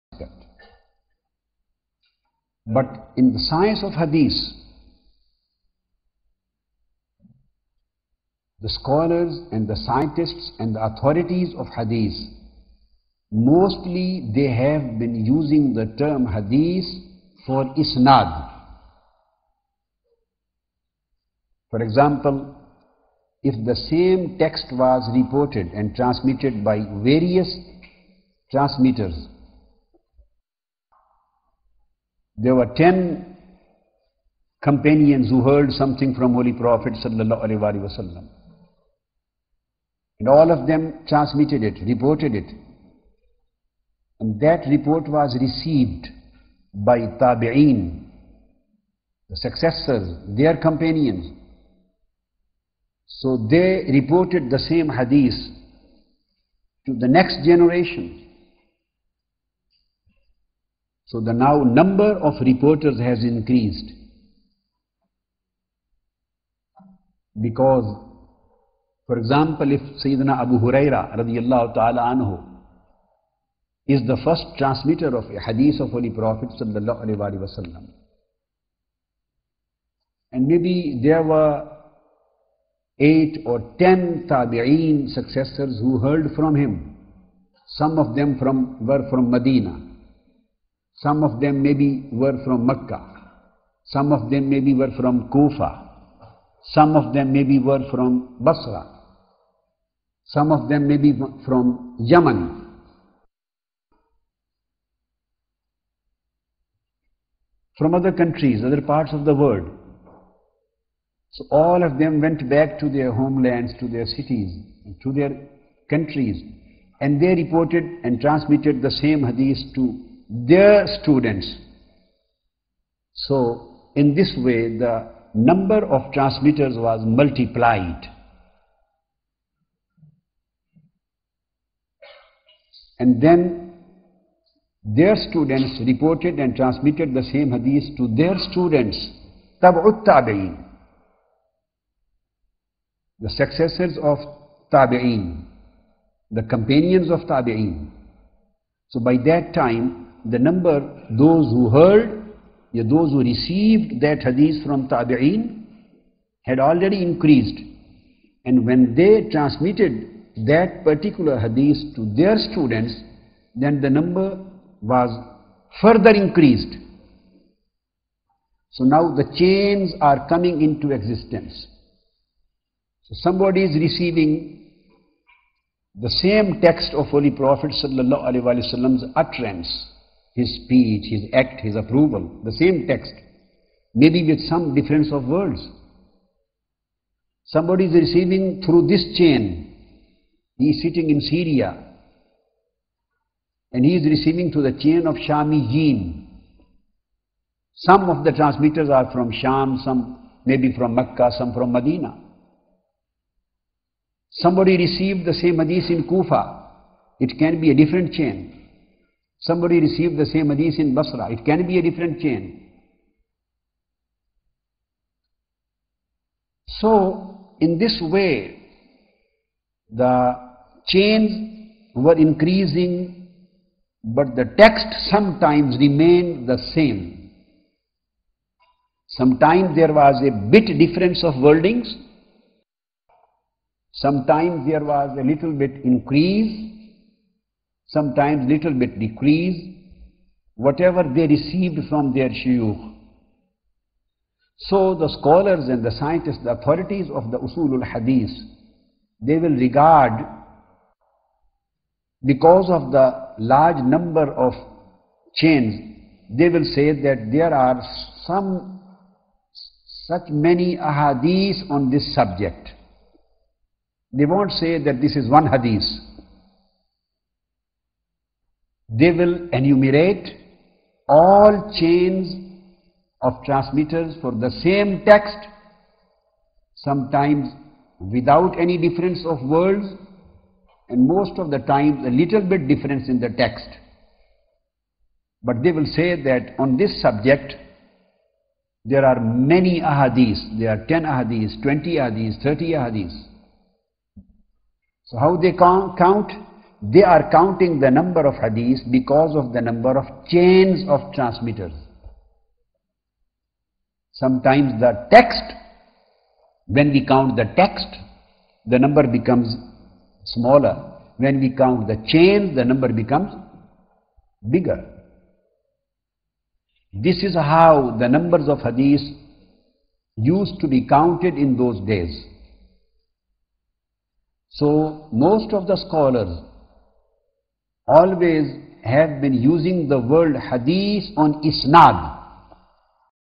Most of the Scholars have been using the word Hadith for Isnad
Allahumma Salli Ala Sayyidina Wa Maulana Muhammadin Wa Aala Alihi Wa Sahbihi Wa Barik Wa Salim.
Assalatu Was Salaamu Alaika Ya Rasool Allah